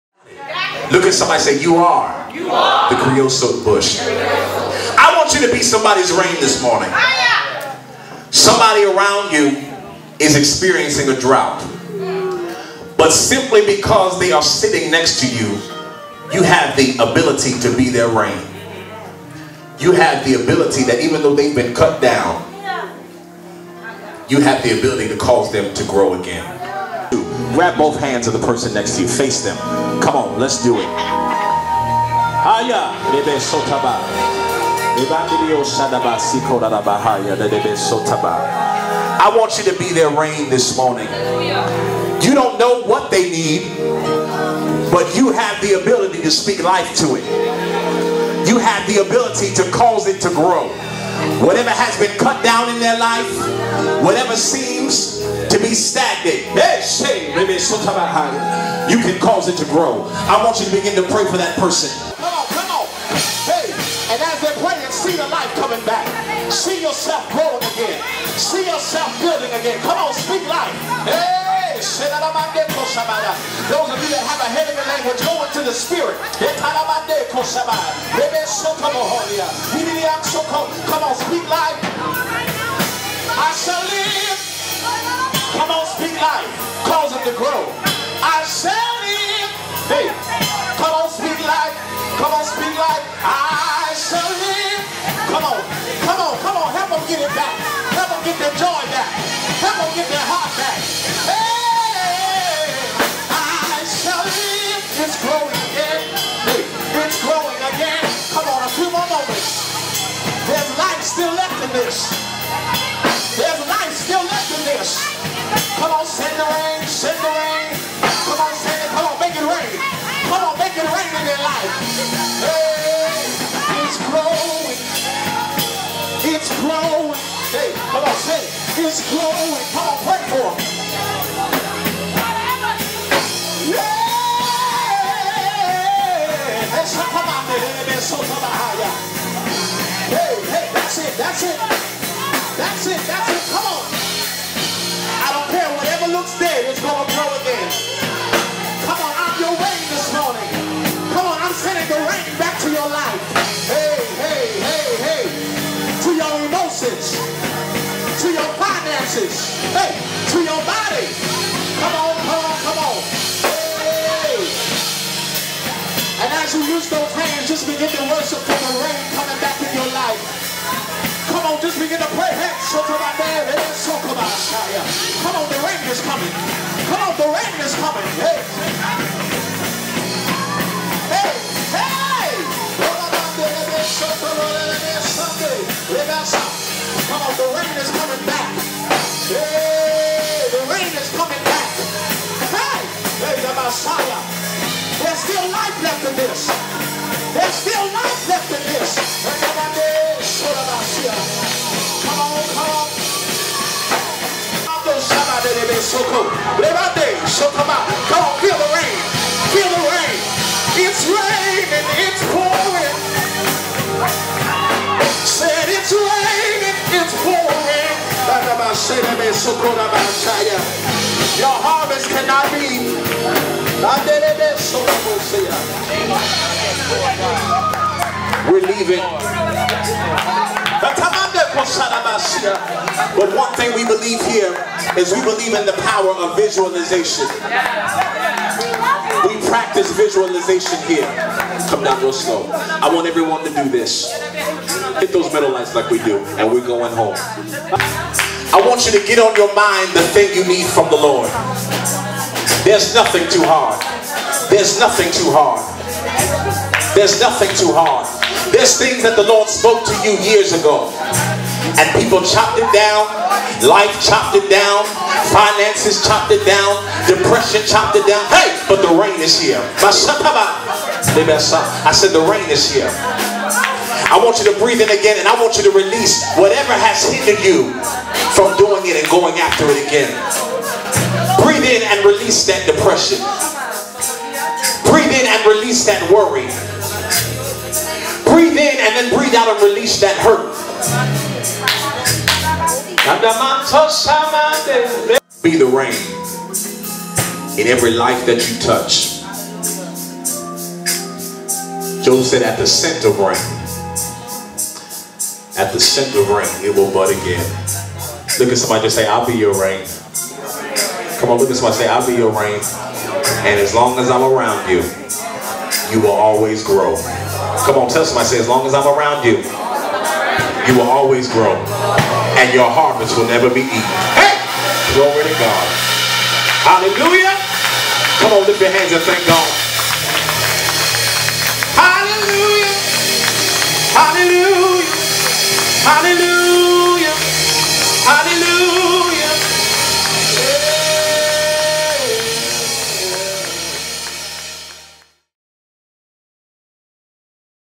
Pastor Cameron Washington - Be The Rain - Worship & Prayer
Enjoy this moment from our Sunday worship after our Pastor Cameron Washington preached a powerful message on being the rain to help someone else grow.